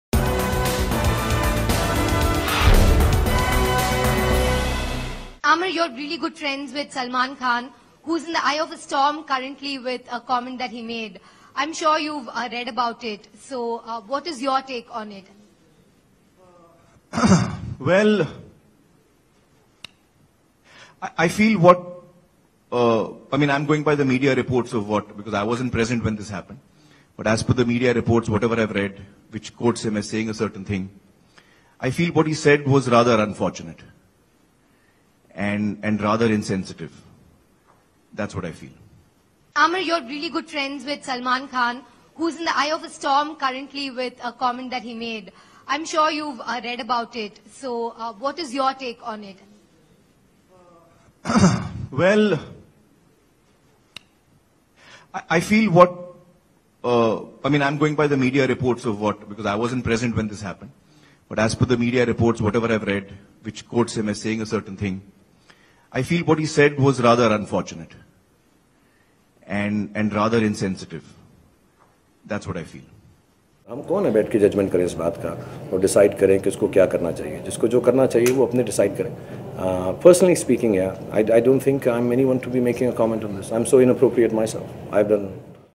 Aamir Khan Responds On Salman Khan's abduction Woman Remarks | Calls The Remarks as Insensitive
Bollywood Mr Perfectionist Aamir Khan Responded on his Close Friend Salman Khan's abduction Woman Remarks, Where he Called the Remarks as Insensitive and Unfortunate. Check Out The Video.